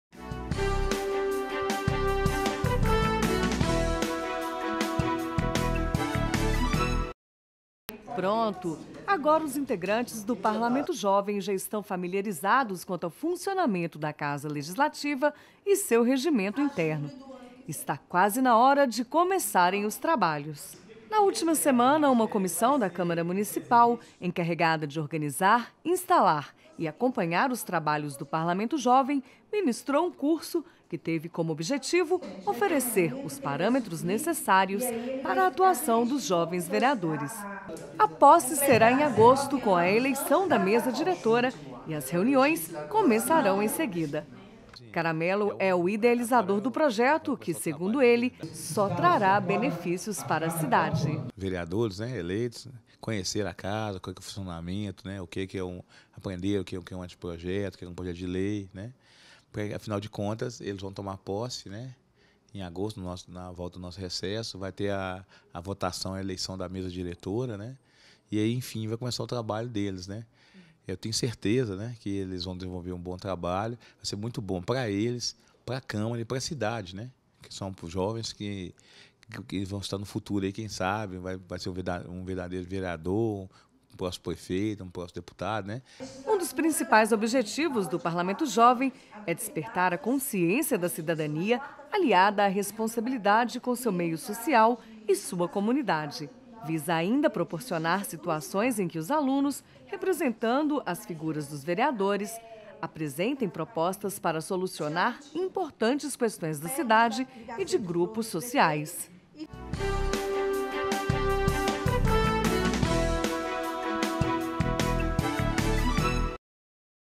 LI CAPACITAÇÃO PARLAMENTO JOVEM 26 06 18 HD
Vereadores do Parlamento Jovem se capacitam antes do início das sessões.